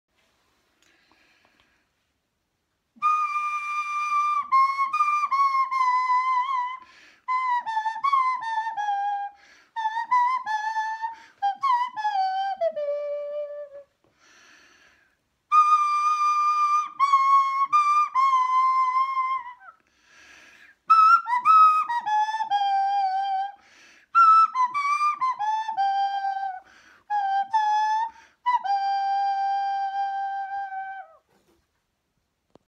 Post Liams Rescue
Following the rescue of my friend Liam from my virtual garden river. ( see previous video) He is so happy (as you can see), that he has invited me to join the band with me on the nose flute